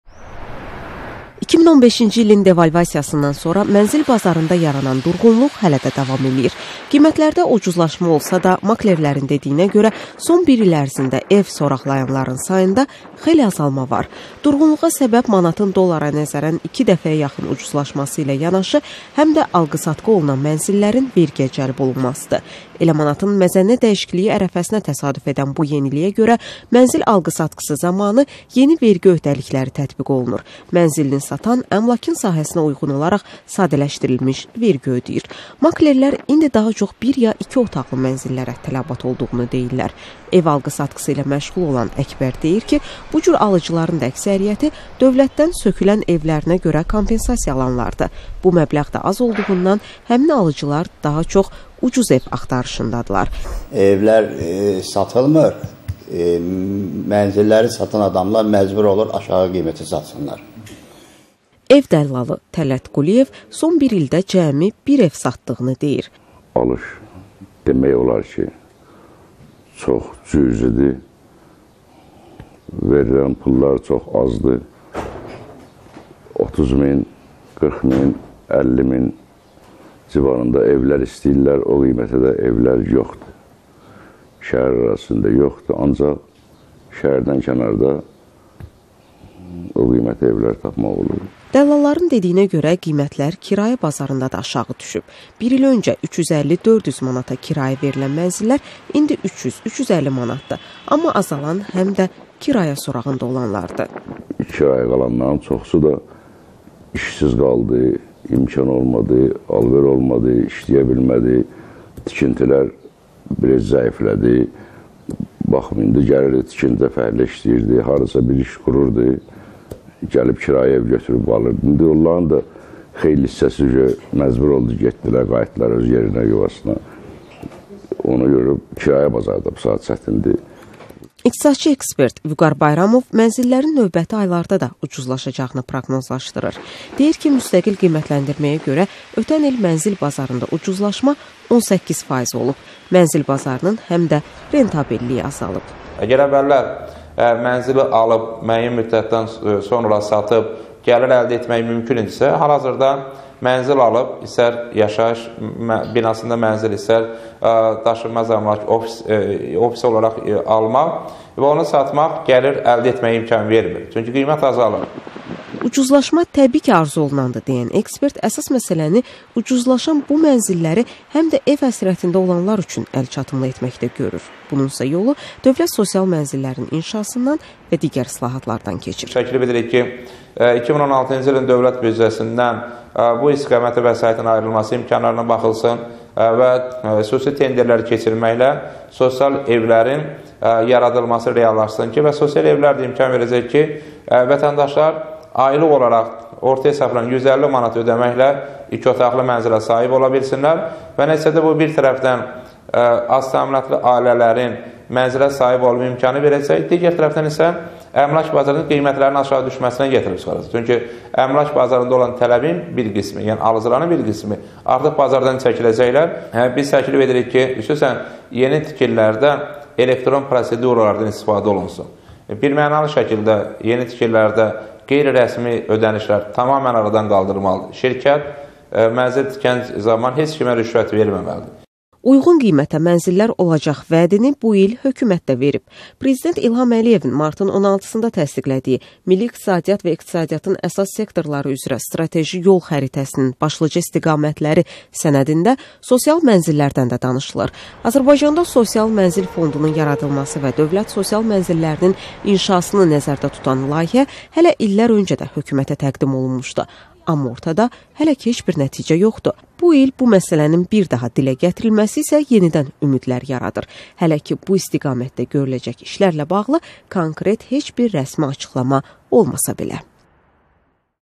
Evlərin qiyməti düşüb, yoxsa qalxıb? - [cavab]
2015-ci ilin devalvasiyasından sonra mənzil bazarında yaranan durğunluq hələ də davam edir. Qiymətlərdə ucuzlaşma olsa da, maklerlərin dediyinə görə, son bir il ərzində ev soraqlayanların sayı xeyli azalıb. Ekspertlər isə mənzillərin bundan sonra da ucuzlaşacağını proqnozlaşdırır.